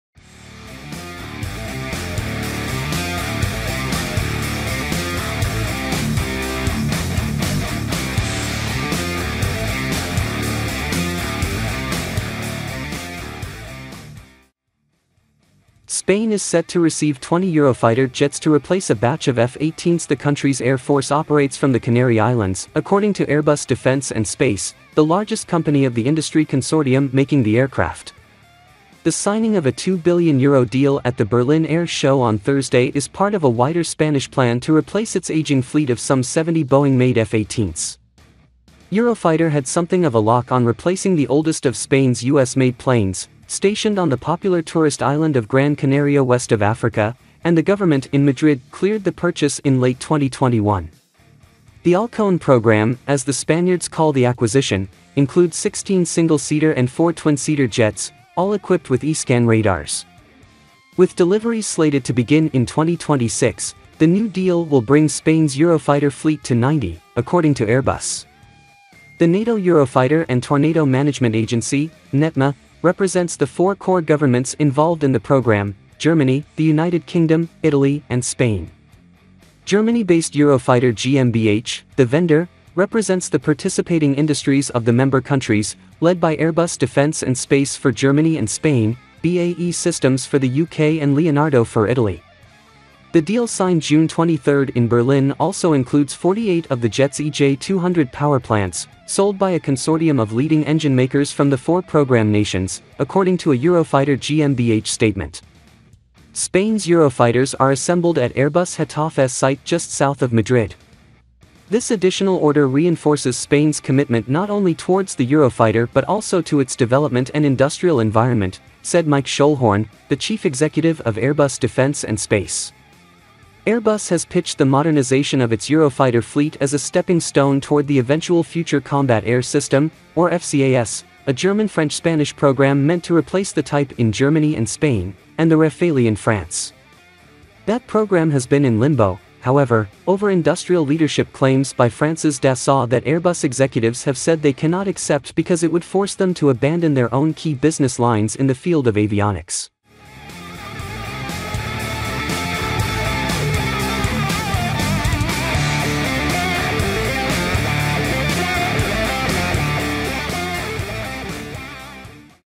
Spain finalizes $2.1 billion deal for 20 Eurofighters to replace old F-18s
Spain is set to receive 20 Eurofighter jets to replace a batch of F-18s the country’s air force operates from the Canary Islands, according to Airbus Defence and Space, the largest company of the industry consortium making the aircraft.

The signing of a €2 billion ($2.1 billion) deal at the Berlin Air Show on Thursday is part of a wider Spanish plan to replace its aging fleet of some 70 Boeing-made F-18s. Eurofighter had something of a lock on replacing the oldest of Spain’s U.S.-made planes, stationed on the popular tourist island of Gran Canaria west of Africa, and the government in Madrid cleared the purchase in late 2021.

The Halcon program, as the Spaniards call the acquisition, includes 16 single-seater and four twin-seater jets, all equipped with e-scan radars. With deliveries slated to begin in 2026, the new deal will bring Spain’s Eurofighter fleet to 90, according to Airbus.

The NATO Eurofighter and Tornado Management Agency (NETMA) represents the four core governments involved in the program: Germany, the United Kingdom, Italy and Spain. Germany-based Eurofighter GmbH, the vendor, represents the participating industries of the member countries, led by Airbus Defence and Space for Germany and Spain, BAE Systems for the U.K. and Leonardo for Italy.

The deal signed June 23 in Berlin also includes 48 of the jets’ EJ200 power plants, sold by a consortium of leading engine makers from the four program nations, according to a Eurofighter GmbH statement.

Spain’s Eurofighters are assembled at Airbus’ Getafe site just south of Madrid.

“This additional order reinforces Spain’s commitment not only towards the Eurofighter but also to its development and industrial environment,” said Mike Schoellhorn, the chief executive of Airbus Defence and Space.”

Airbus has pitched the modernization of its Eurofighter fleet as a stepping stone toward the eventual Future Combat Air System, or FCAS, a German-French-Spanish program meant to replace the type in Germany and Spain, and the Rafale in France.

That program has been in limbo, however, over industrial leadership claims by France’s Dassault that Airbus executives have said they cannot accept because it would force them to abandon their own key business lines in the field of avionics.